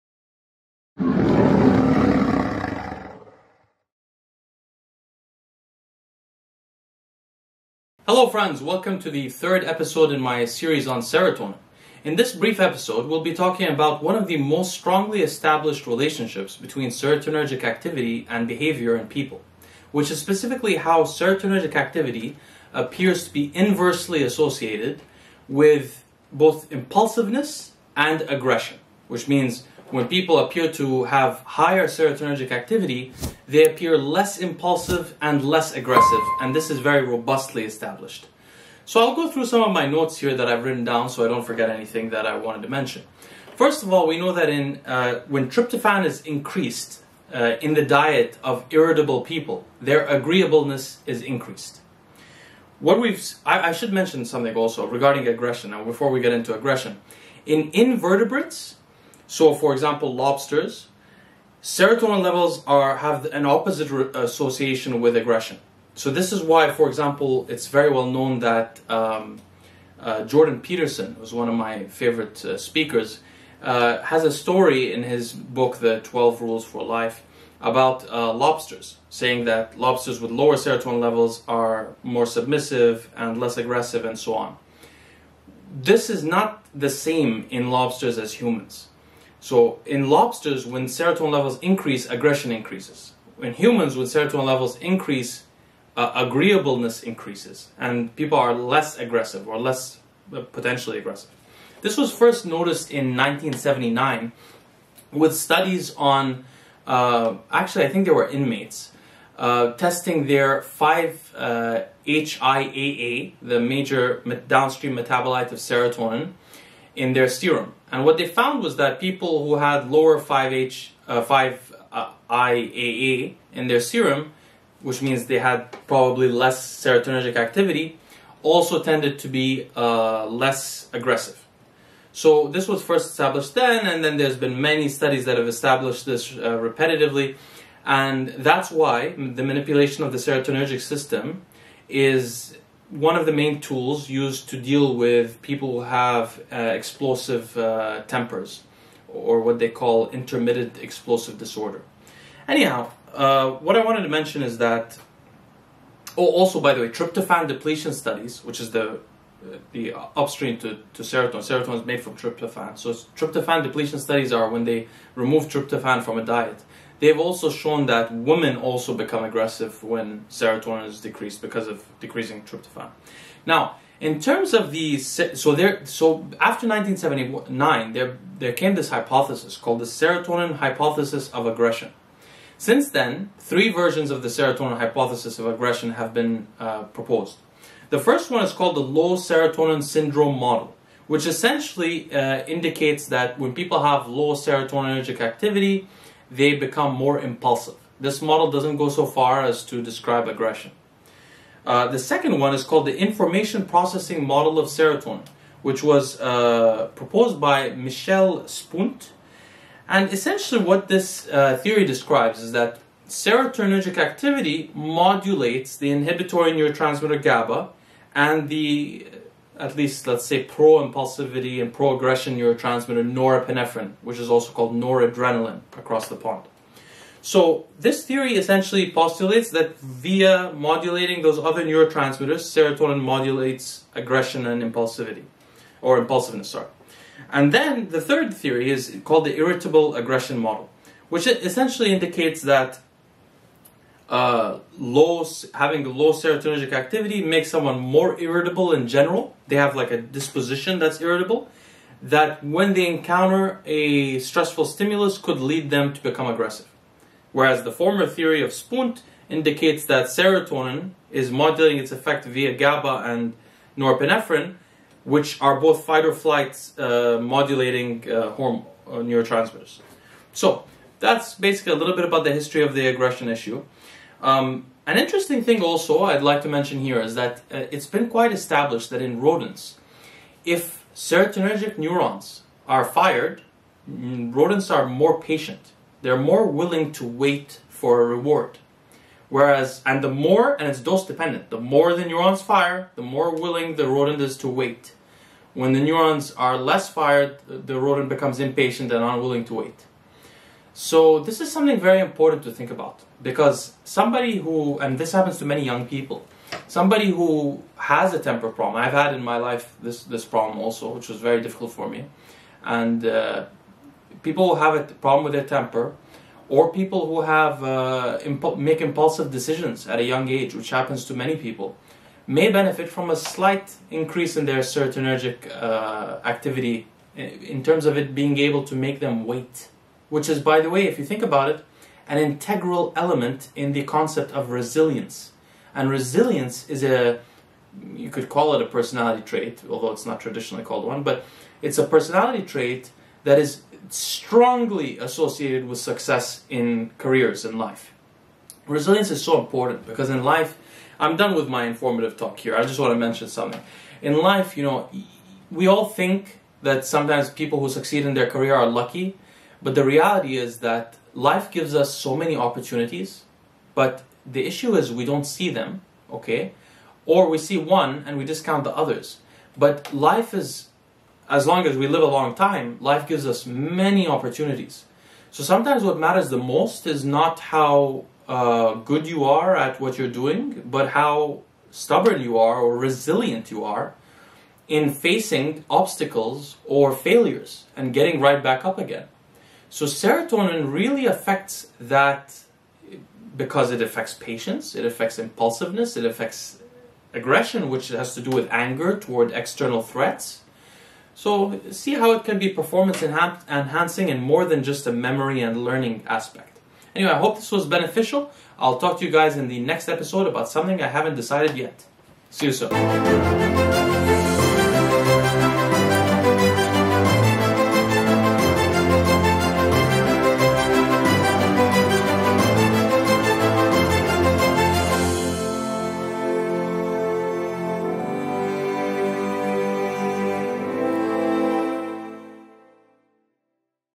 How Serotonin Reduces Impulsiveness and Aggression (The Serotonergic Series #3)
In this video, I discuss how serotonin reduces both impulsiveness and aggression in people.

🔗 Connect & Explore 🔗
JOIN THE COMMUNITY:
OTHER CHANNELS
Lucie ▶    / @luciec.7692
Kids ▶    / @bilinguini